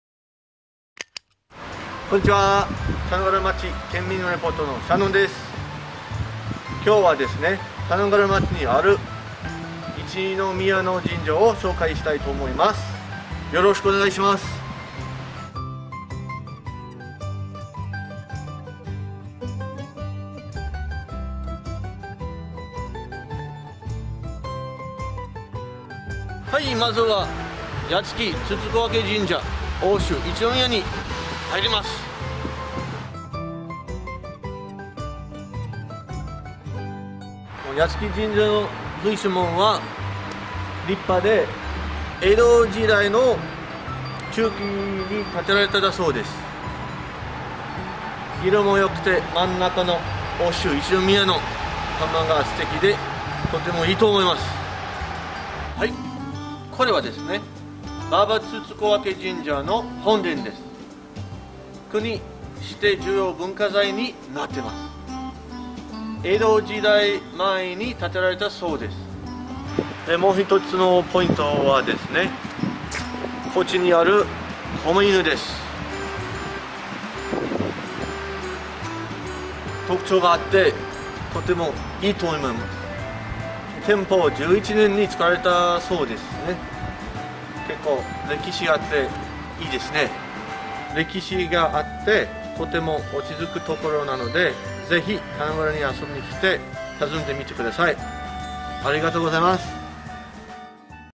ふるさとシェア！棚倉町 国指定重要文化財も登場！神社めぐり
県内59市町村の県民リポーターによるふるさと自慢！

県民リポーターに募集中！！